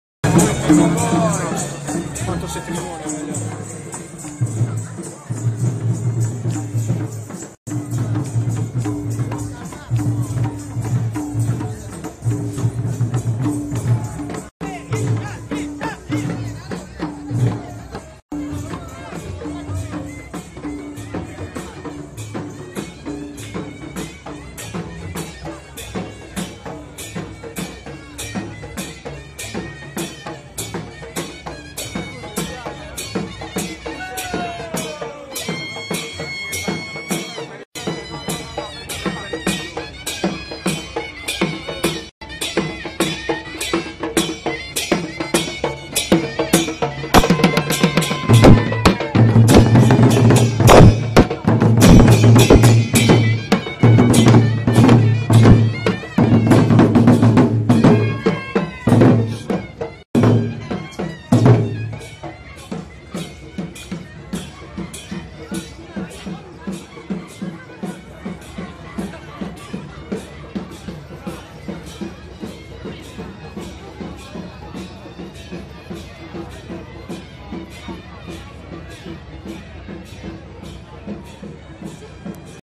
Un pezzo di India a Bari vecchia
Corteo di maschere indiane tra i vicoli del borgo antico di Bari